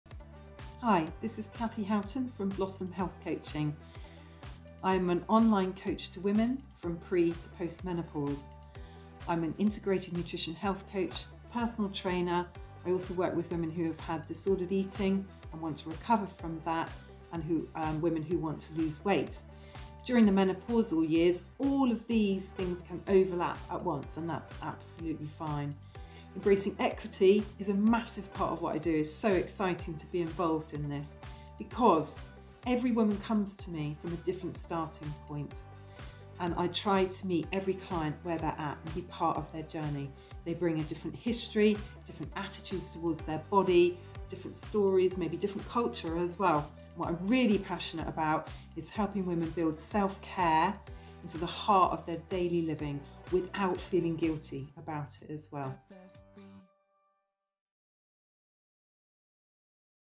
Embrace Equity - Cathy Houghton
Day 2 of our EmbraceEquity FSBmember series.Ahead of InternationalWomensDay 2023, we’ve collaborated with a handful of inspiring FSB members to share their tips on embracing equity in the workplace. Today, meet Cathy Houghton from Blossom Health Coaching.